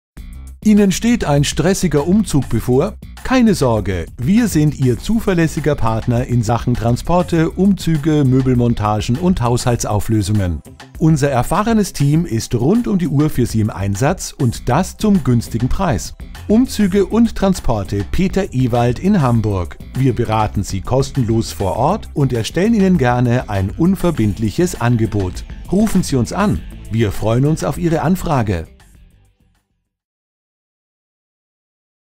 Büroumzüge Hamburg Umzüge + Transporte Peter Ewald
Umzüge + Transporte Peter Ewald in Hamburg ist Ihre erste Wahl, wenn Sie nach Büroumzüge oder Transporte suchen. Für mehr Informationen erreichen Sie uns unter der Telefonnummer 9 oder schauen Sie direkt vor Ort vorbei: Hesterlanden 7, 22175 Hamburg. Besuchen Sie uns auch unter der oben genannten Webadresse!